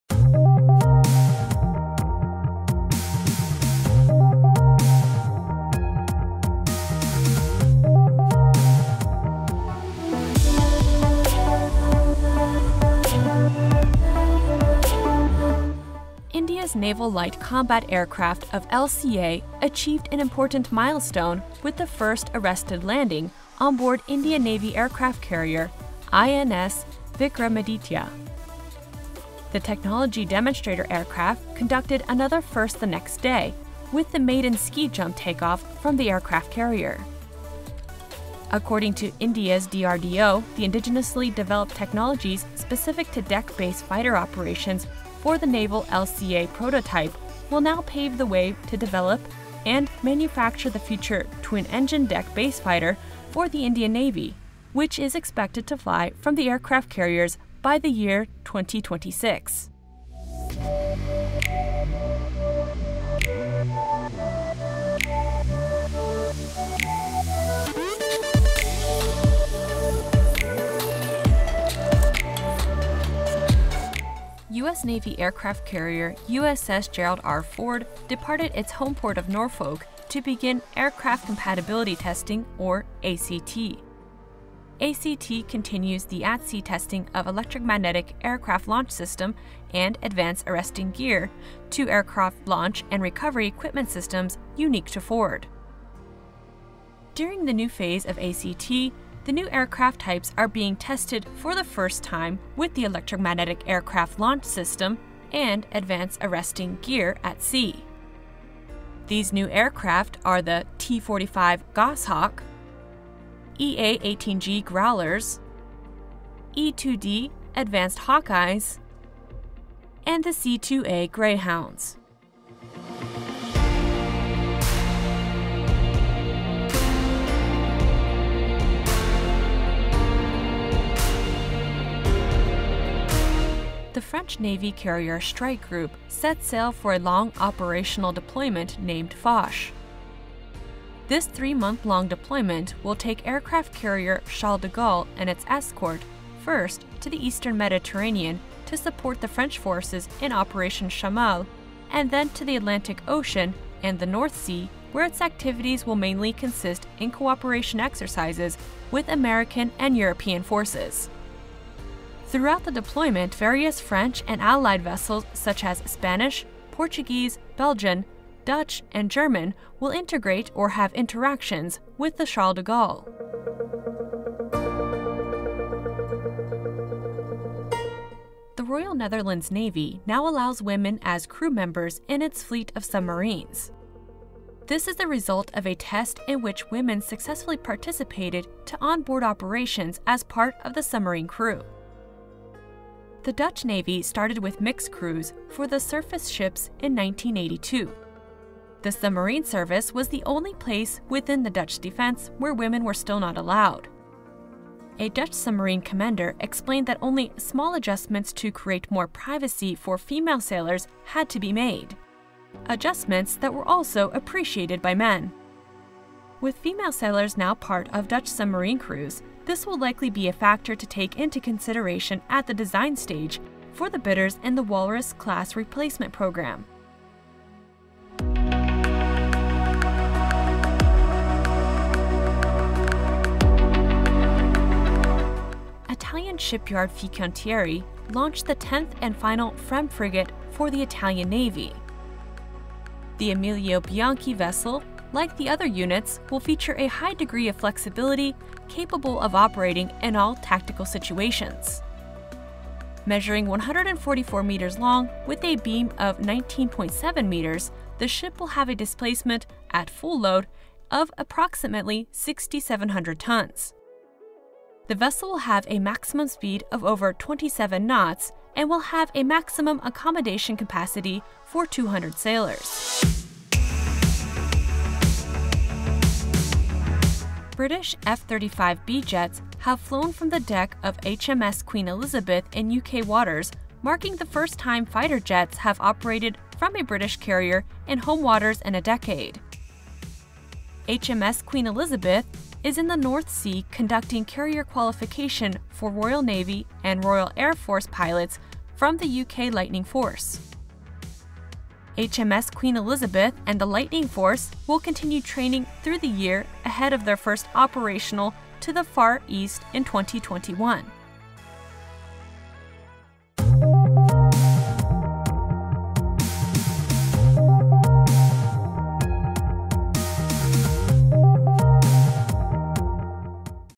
Naval News Monthly Recap - January 2020
Here is NavalNews.com's first Monthly Recap of 2020!

This month, aircraft carriers are a prominent topic in our coverage. We also cover females in submarine service and the last FREMM launch in Italy.

00:17 - India's Naval LCA lands aboard & take off from aircraft carrier for the first time

01:00 - Aircraft carrier USS Gerald R. Ford conducts aircraft compatibility testing

02:04 - French Navy carrier strike group, aircraft carrier Charles de Gaulle begin ‘Foch’ deployment

02:53 - Royal Netherlands Navy now admits female sailors for submarine service

03:48 - Fincantieri launched the Italian Navy’s tenth & final FREMM frigate

04:33 - British F-35B Lightning jets fly from HMS Queen Elizabeth in UK waters